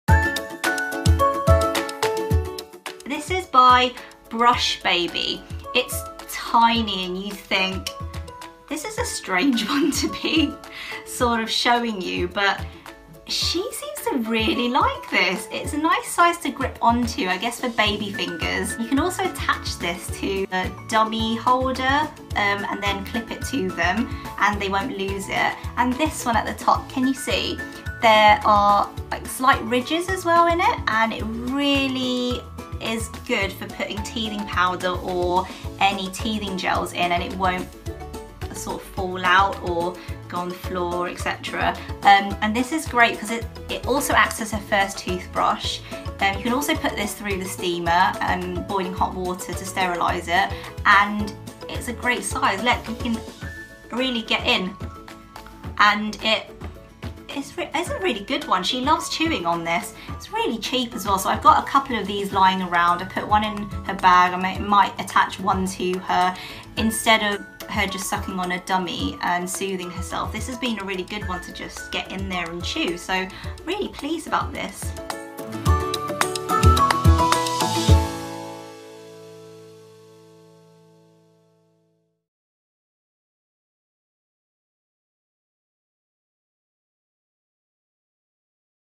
Brush-Baby Chewable Toothbrush review
Mum Nicole talks about the Brush-Baby Chewable Toothbrush which cleans baby's gums as they chew. Great for teething.